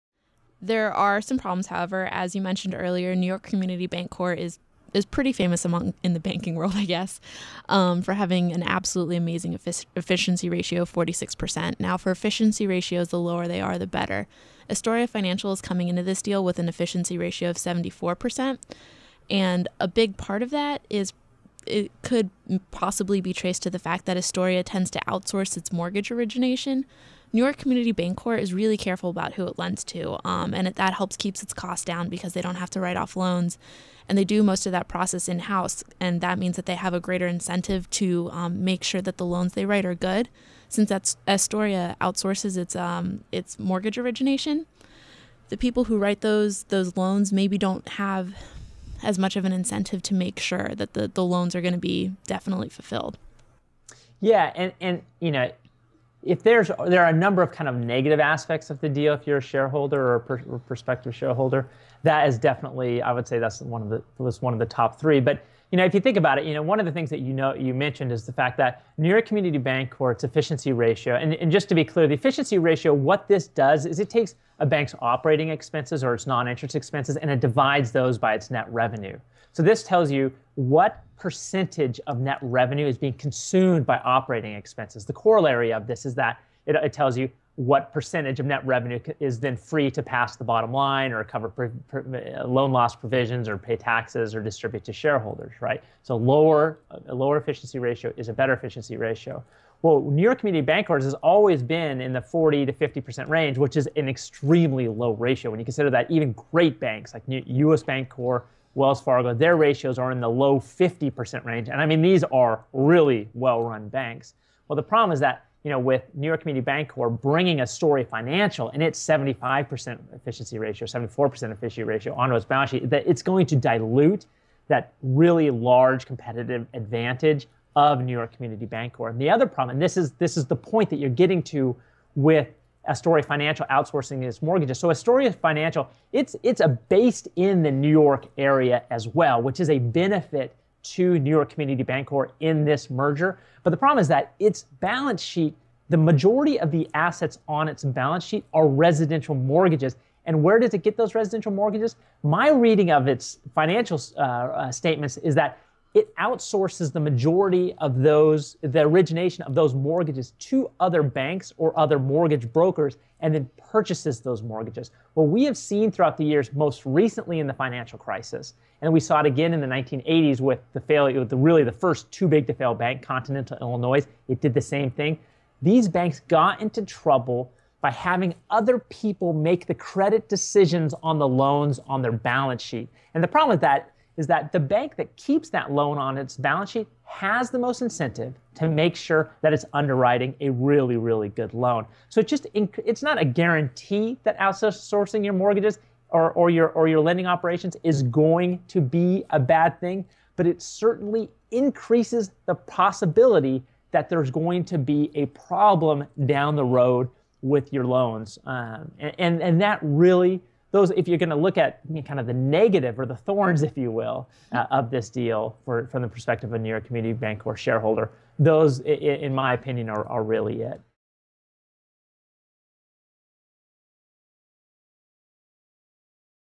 Potential Problems with NYCB Adding Astoria Financial to Its Wagon Train
While overall the merger between New York Community Bancorp and Astoria Financial appears to be successful, challenges may yet lie ahead for NYCB. Astoria Financial’s high efficiency ratio could dull NYCB’s edge over its competition.

This podcast was recorded on 11/9/2015.